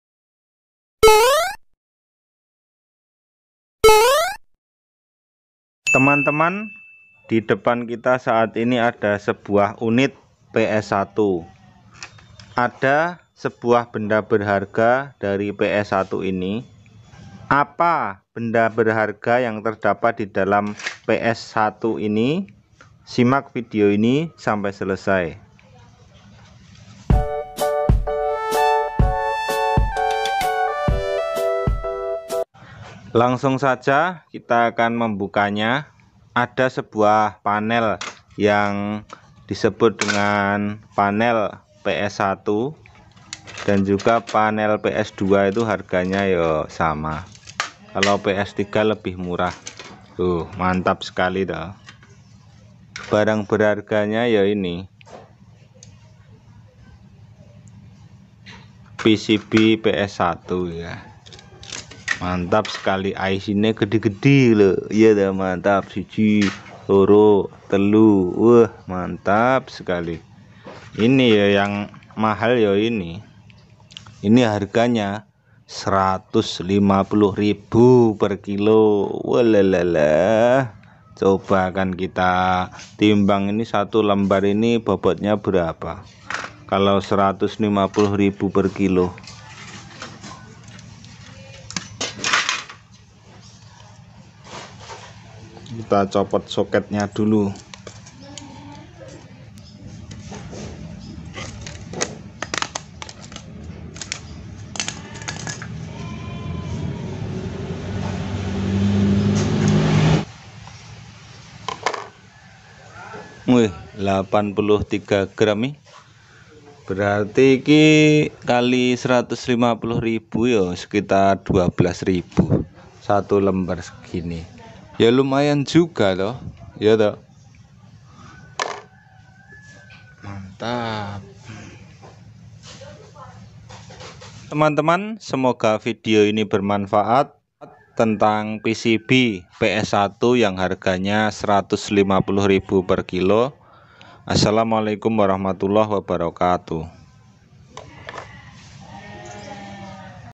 Wow.. Inilah Benda Berharga Di Balik PS 1| Rahasia bisnis rosok.
Ada sebuah panel
Di dalam PS 1
Yang harganya cukup tinggi..
Berapa harganya..
Yuk simak videonya ya..